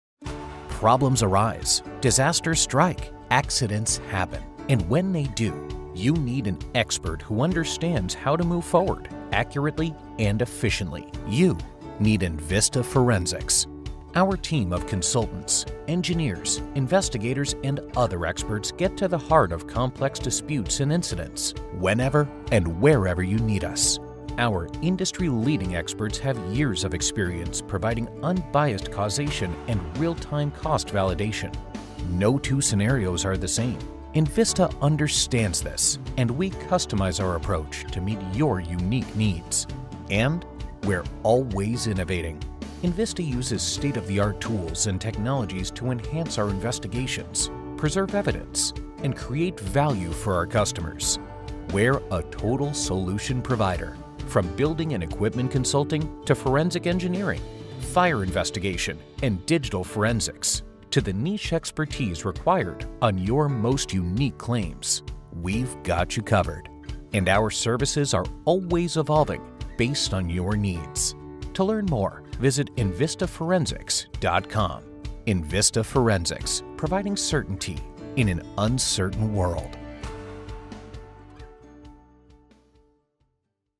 We Solve Complex Problems for our Clients in the Face of Disaster | Envista Forensics
Envista Forensics is made of highly trained experts, engineers and consultants providing investigative and analysis services on WHY a loss happened. From the most common routine requests that require rapid or local assistance to complex losses that require the need for multi-disciplinary management and experience, to rarer and more unusual cases that need a highly specialized, niche industry expert--our passion lies in the science behind the why, and providing that education to our clients.